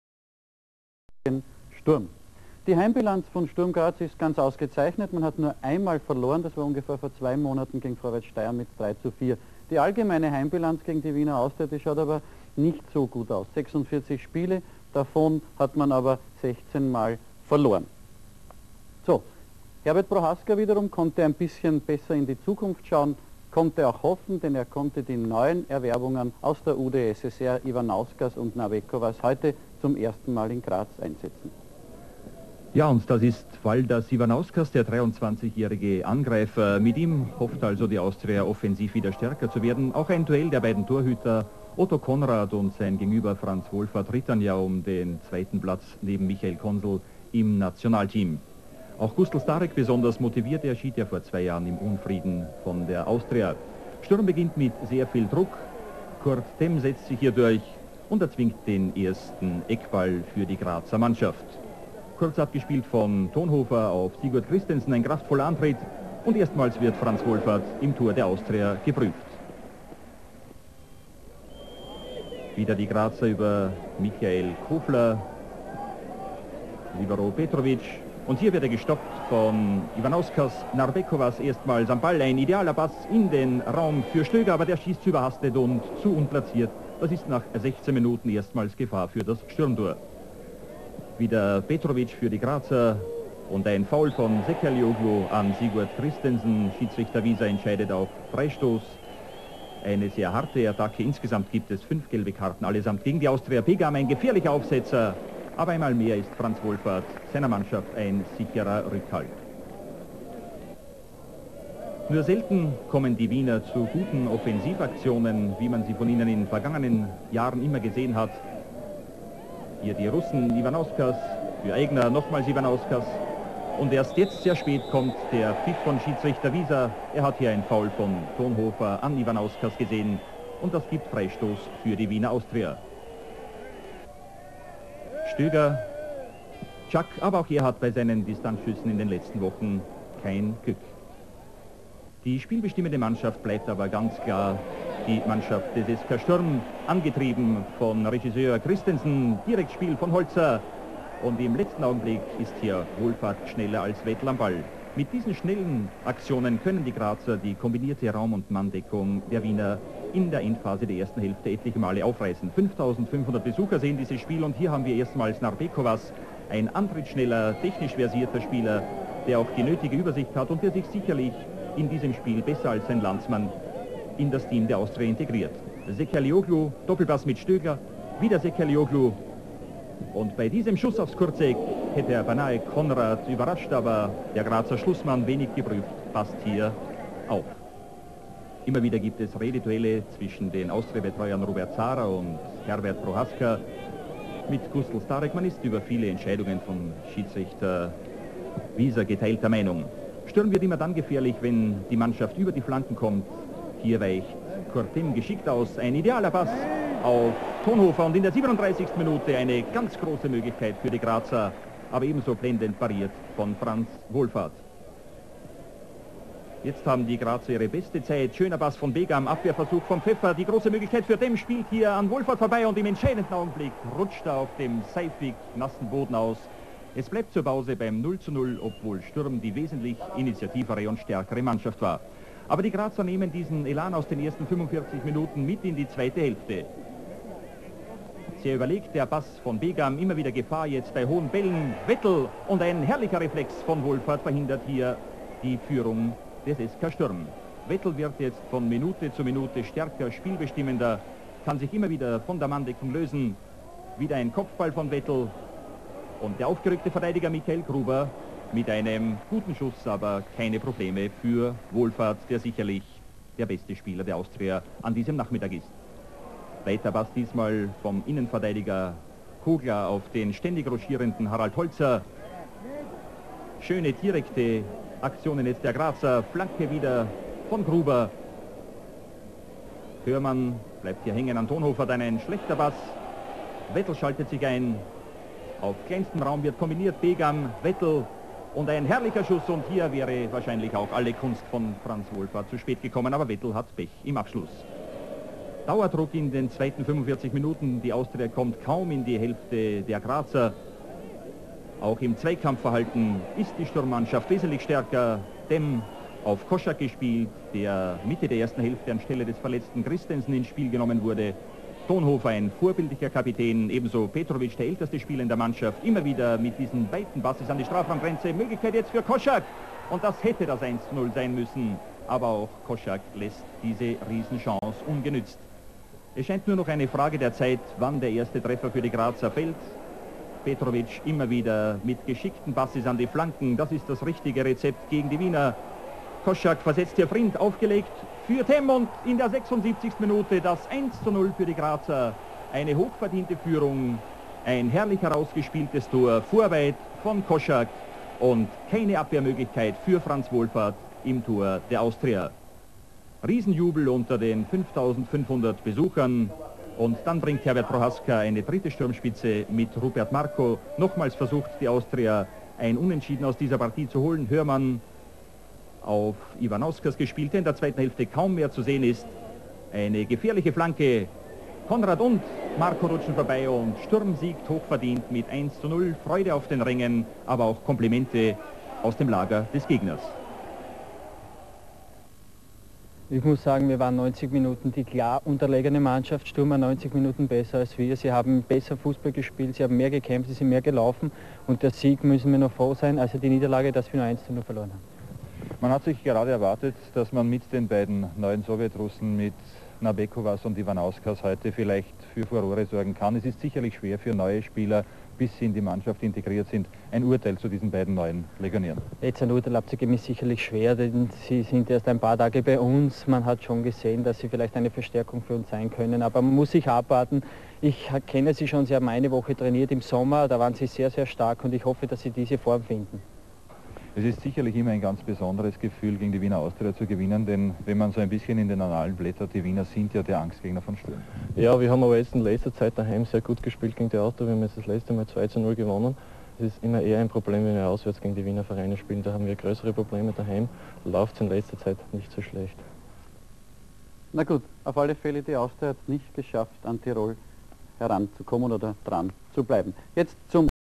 Sturm Graz - Austria Wien 1:0 - Saison 1990/91
Sturm Graz Samstag, 10. November 1990 Austria Wien
Sturm Graz
1:0
Austria Wien

Tore
1 : 0 Kurt Temm 75.

1 Otto Konrad
Michael Kofler
Walter Kogler
10 Mihailo Petrović
Sigurd Kristensen 33'
Michael Gruber
Dietmar Pegam
5 Harald Holzer
6 Kurt Temm
6 Heinz Thonhofer  gelb 87'
15 Arnold Wetl 89'
Reservespieler
Josef Koch 89'
11 Günther Koschak 33'
1 Franz Wohlfahrt
Robert Frind  gelb 12'
Ernst Aigner  gelb 70'
3 Anton Pfeffer
4 Attila Sekerlioglu 80'
Walter Hörmann  gelb 34'
5 Manfred Zsak  gelb 80'
8 Arminas Narbekovas
10 Peter Stöger
14 Hannes Pleva
18 Valdas Ivanauskas  gelb 60'
Reservespieler
Rupert Marko 80'

Trainer: August Starek Trainer: Herbert Prohaska

Stadion Sturm-Platz Gruabn (Graz / Österreich)
Zuschauerzahl 5.000
Schiedsrichter Alfred Wieser (Österreich)
Schiedsrichter-Assistent Karl Hitzenhammer (Österreich)
Schiedsrichter-Assistent Anton Kuri (Österreich)